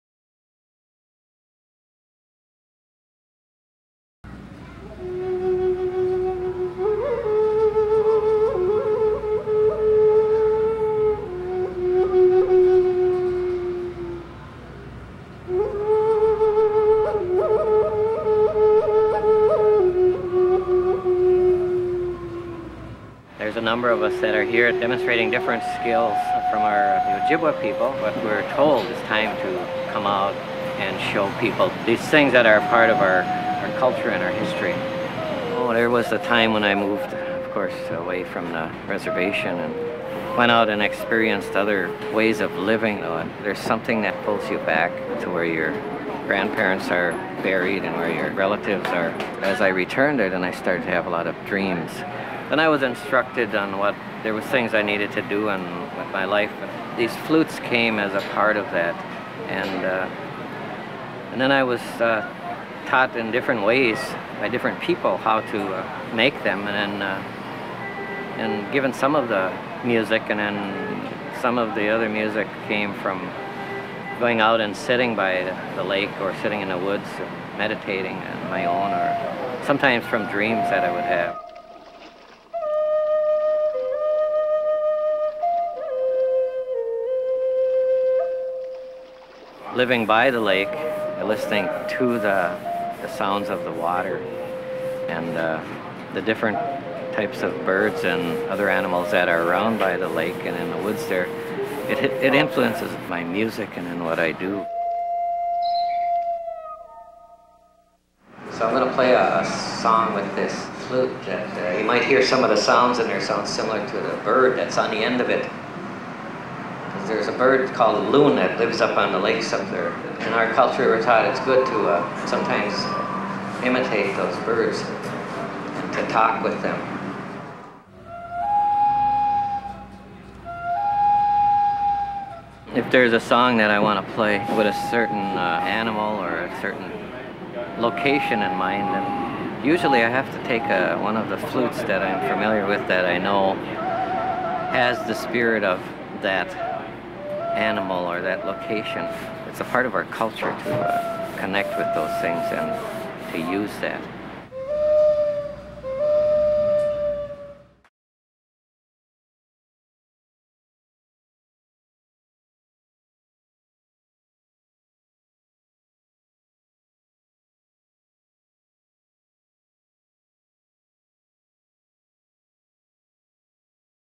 Frank 'Anakwad' Montano plays and describes his flutes
Frank Montano performs and discusses how he came to make flutes and why in this short clip recorded during the 1998 Smithsonian Folklife Festival and edited for "Wisconsin Folks."